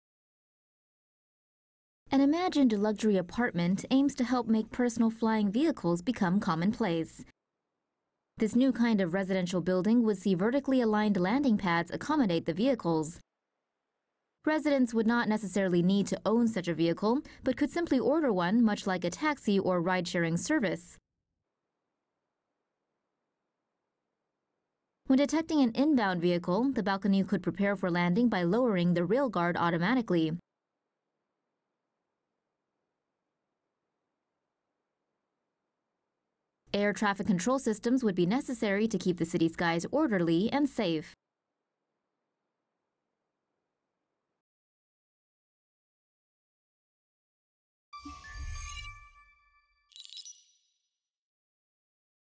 Future apartments will have balconies available for personal flying drones
RESTRICTIONS: NONE

Canadian engineer Charles Bombardier and Indian industrial designer Ashish Thulkar developed the idea of luxury apartments with landing pads for personal fliers.

The concept is one instance of the kind of infrastructure that would need to be developed for personal flying vehicles to become commonplace. This new kind of residential building would see vertically aligned landing pads accommodate the vehicles.

Residents would not necessarily need to own such a vehicle, but could simply order one, much like a taxi or ride-sharing service. When detecting an inbound vehicle, the balcony could prepare for landing by lowering the rail guard automatically.

Bombardier added that there would be a need for air traffic control systems to keep the city skies orderly and safe.

Though the idea is still in it’s infancy, Bombardier and Thulkar believe it may not be long before we see apartment buildings built to code for personal fliers.

RUNDOWN SHOWS:
1. Show human-sized personal flier
2. Drone tower in urban city
3. Personal fliers can be used as a taxi
4. Apartment prepare for personal fliers to land
5. Passengers take the personal flier for a ride
6. Encountering traffic
7. A need for air traffic control for personal fliers

VOICEOVER (in English):

[CUT 1]
An imagined luxury apartment aims to help make personal flying vehicles become commonplace.

[CUT 2]
This new kind of residential building would see vertically aligned landing pads accommodate the vehicles.

[CUT 3]
Residents would not necessarily need to own such a vehicle, but could simply order one, much like a taxi or ride-sharing service.

[CUT 4] [CUT 5]
When detecting an inbound vehicle, the balcony could prepare for landing by lowering the rail guard automatically.

[CUT 6] [CUT 7]
Air traffic control systems would be necessary to keep the city skies orderly and safe.

SOURCES:
WIRED, IMAGINACTIVE